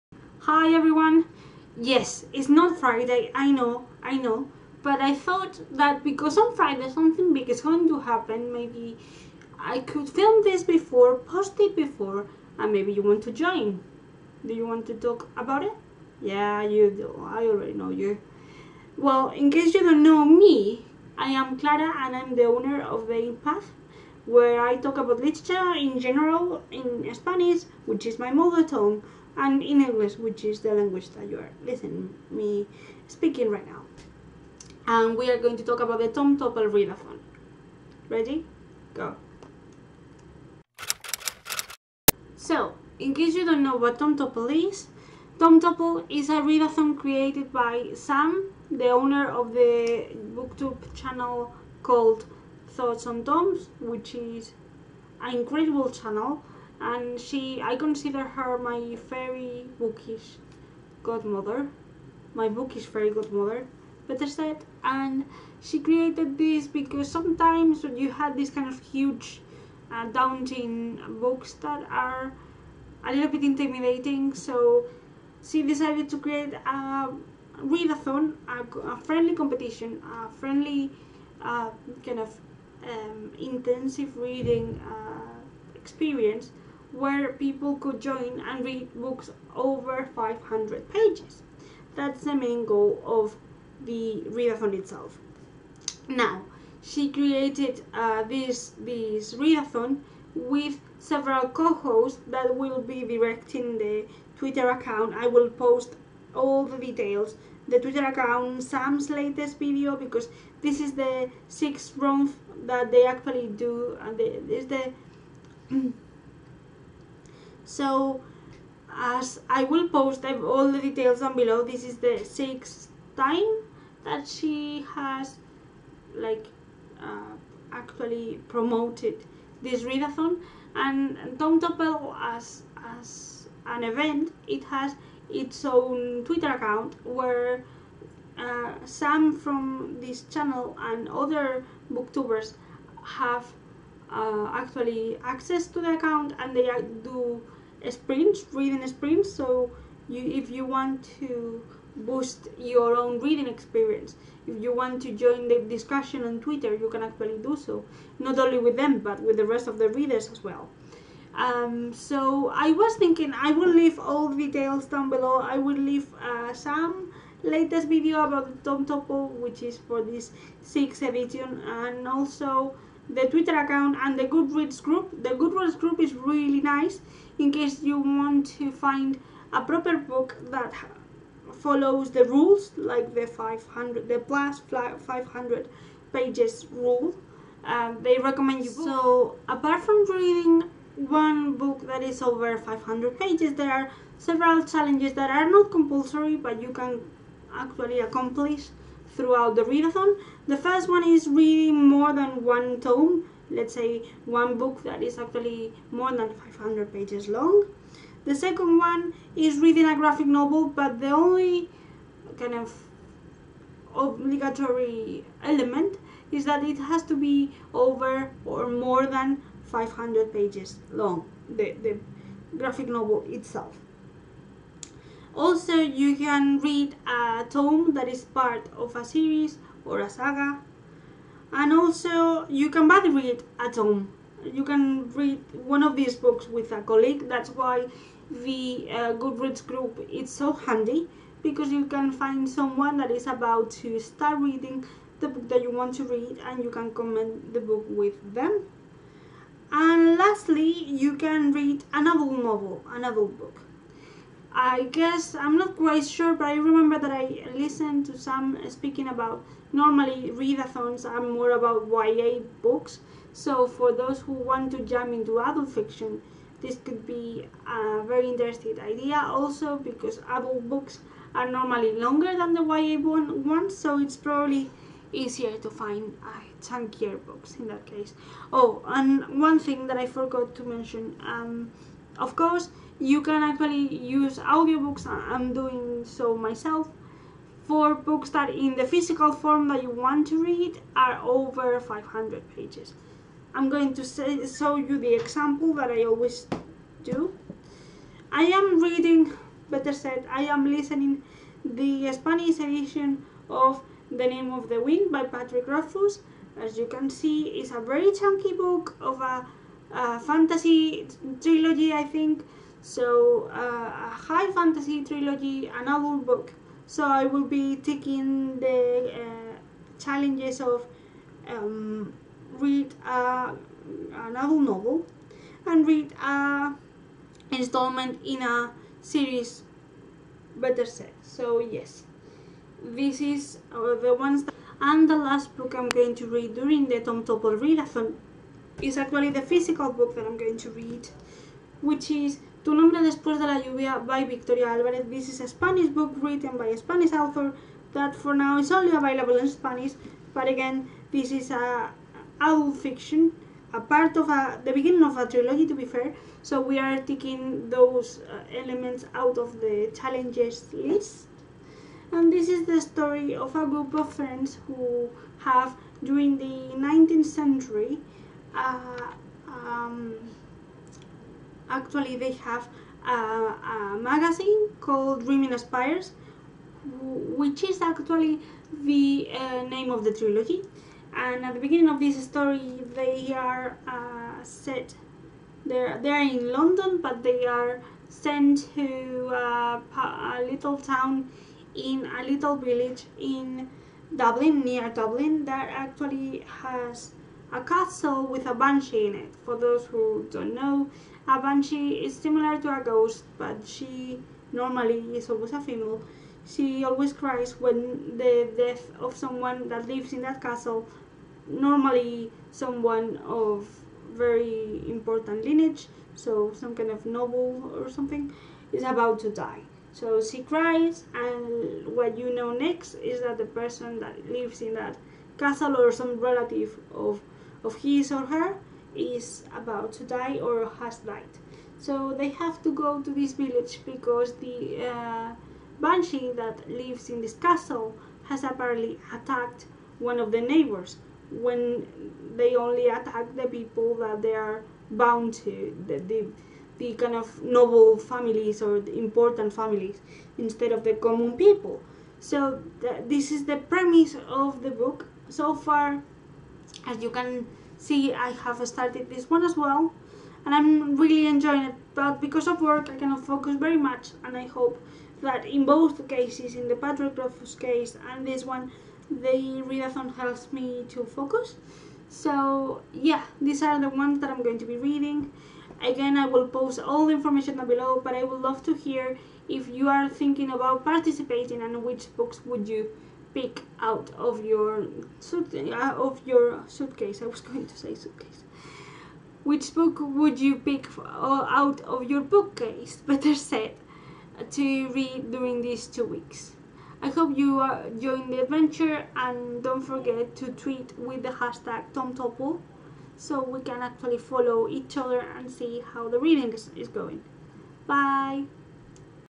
Tome Topple Readathon (6th edition): What it is and My TBR
Hello everyone!

Today I want to do something different... do you want to join me? Welcome to the Tome Topple Readathon

Tome Topple´s Twitter:  @tome_topple

Challenges:
1. Read more than 1 tome
2. Read a graphic novel (still over 500 pages!)
3. Read a tome that is part of a series
4. Buddy read a tome
5. Read an adult novel
** Bonus Challenges: check out my IG for IG challenges closer to the readathon! **

Clara